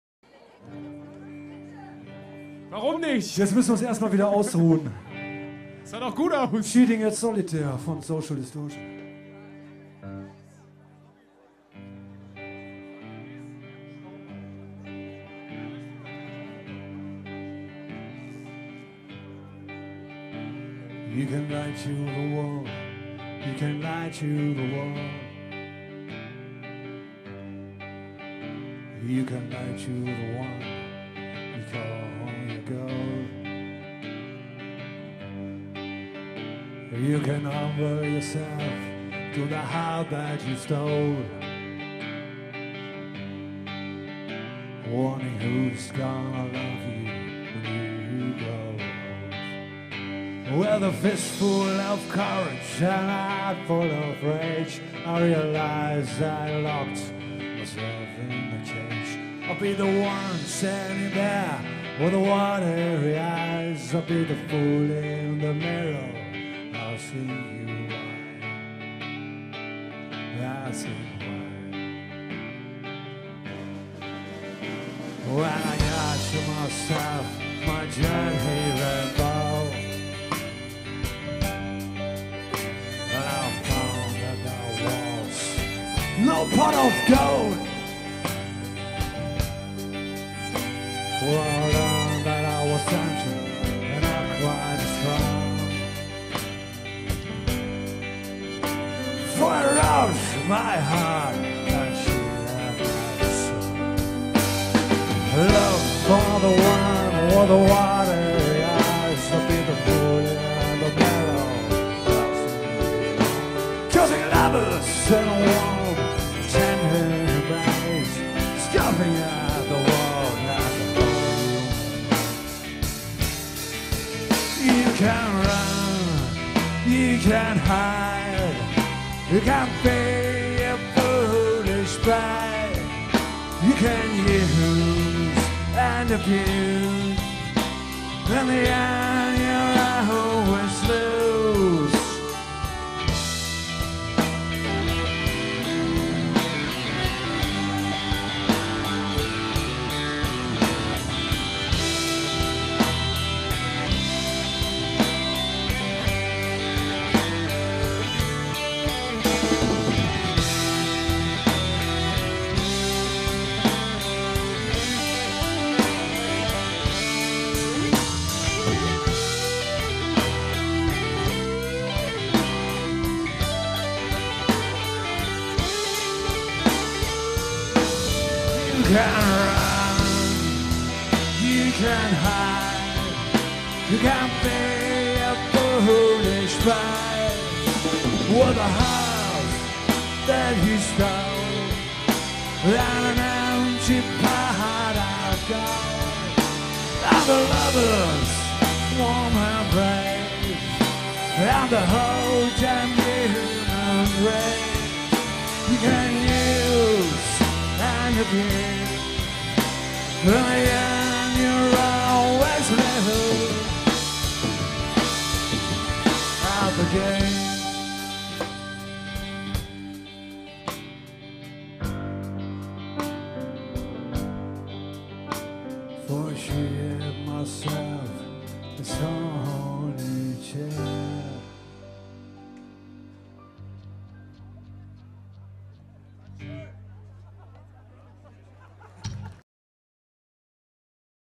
The Porters part 21
Auftritt beim HAMROCK Festival im August 2014 in Hambergen.
Ein Film von Flimmern + Rauschen e.V.